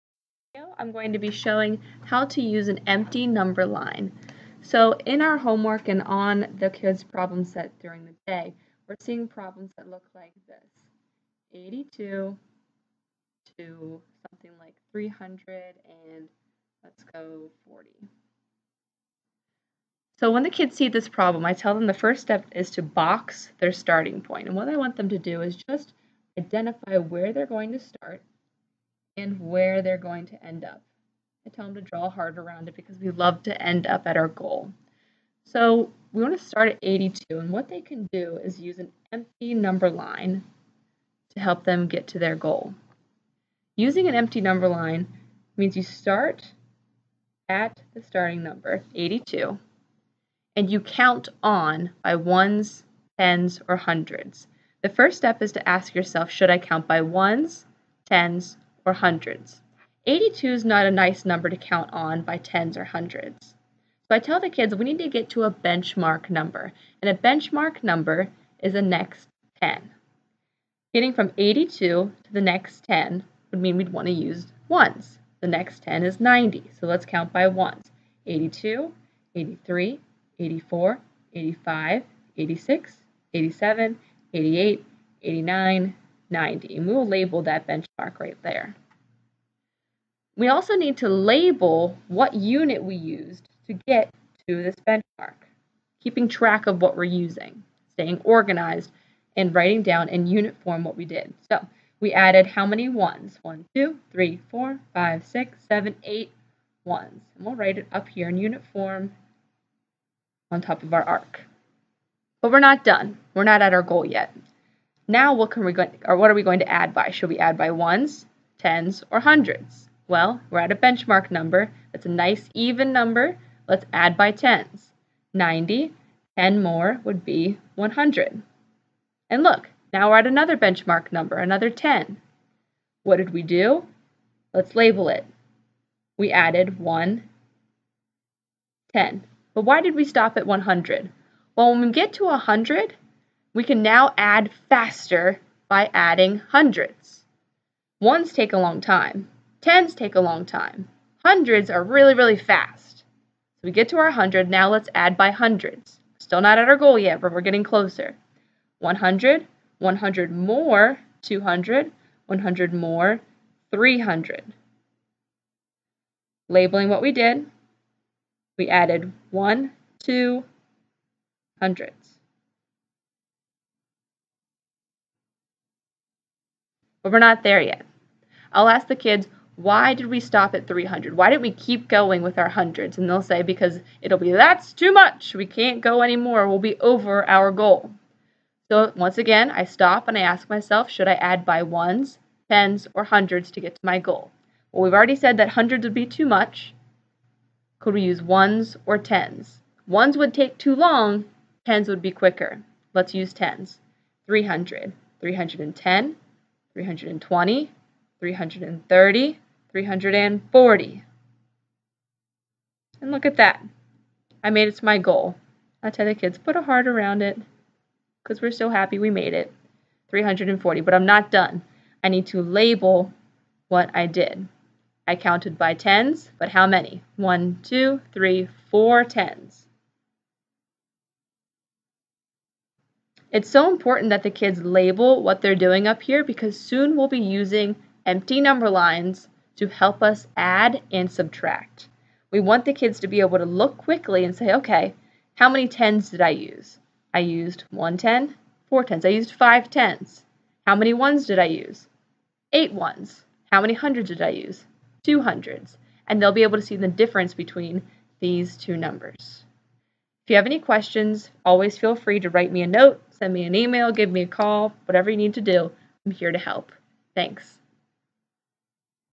Empty Number Line Strategy
We have begun using this strategy to count on by ones, tens, and hundreds. Encourage your students to practice at home!